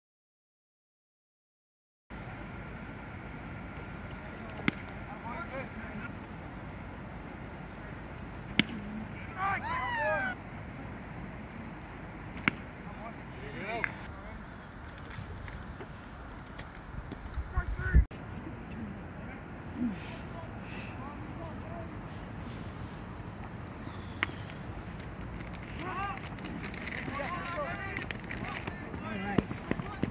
Walla Walla CC 2020 (Covid) Highlights
Season was cancelled due to Covid after 8 games in 2020.